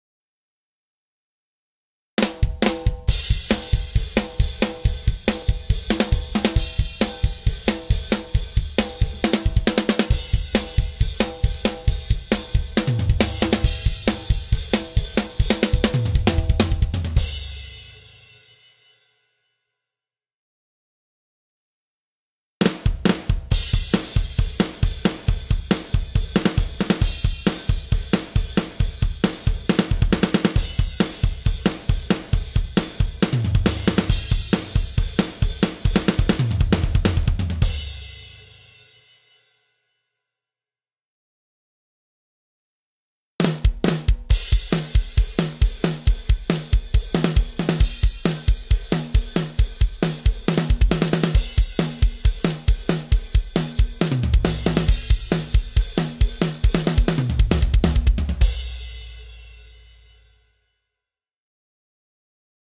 OTW Drums - Rock Drum Sample Replacement
In this video, you'll see Lester Estelle Jr (drummer for Kelly Clarkson and Pillar) play a rock groove on the drums.  You'll hear the original natural drum sound first, followed by the same groove, but with the kick and snare drum sound replaced with various OTW Drums Samples.